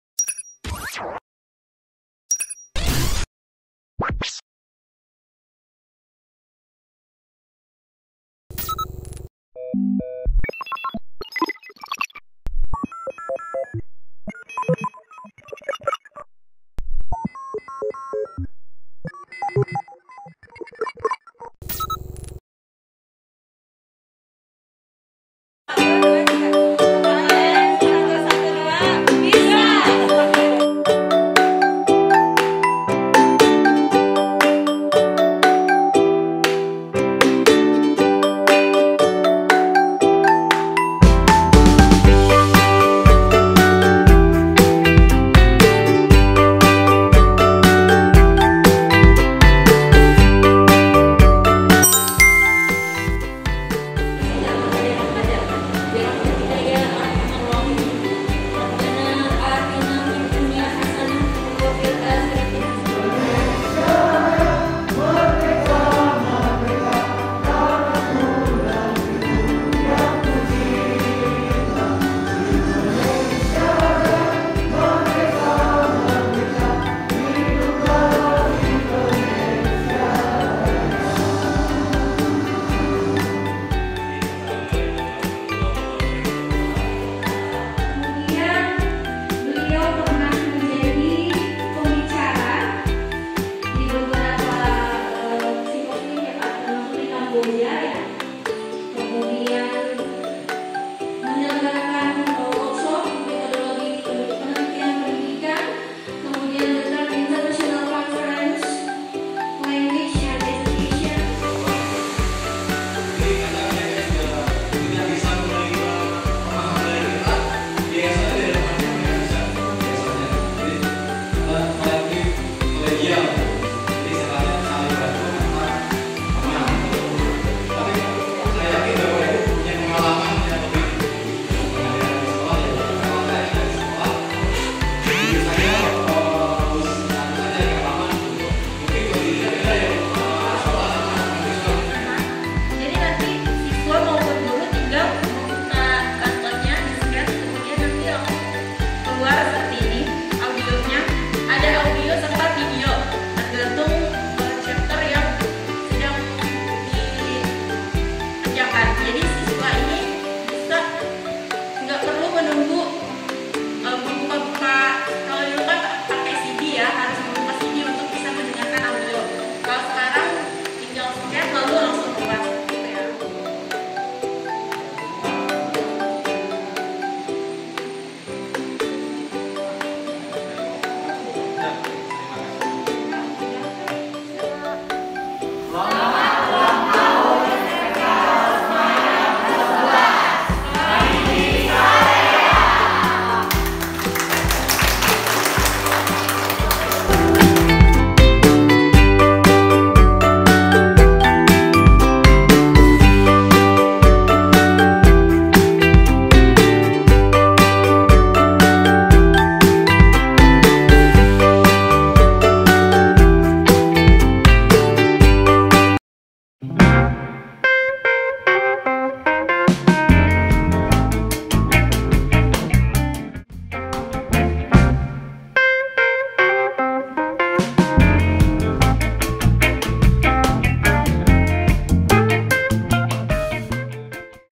112. KKN UNSIKA 2021 - Sosialisasi Modul Bahasa Inggris untuk Jurusan TKJ
Hallo warga maroon!
Berikut merupakan dokumentasi hasil kegiatan KKN UNSIKA 2021 Kelompok 112 : PELATIHAN PENGEMBANGAN MATERI AJAR BAHASA INGGRIS UNTUK PEMBELAJARAN JARAK JAUH
BAGI GURU BAHASA INGGRIS SMK ROSMA KARAWANG

Kontak : (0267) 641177, 641367, 642582

Lembaga Penelitan dan Pengabdian kepada Masyarakat (LPPM)
Universitas Singaperbangsa Karawang
Jl. H.S. Ronggowaluyo Teluk Jambe Karawang, Jawa Barat. Indonesia, 41361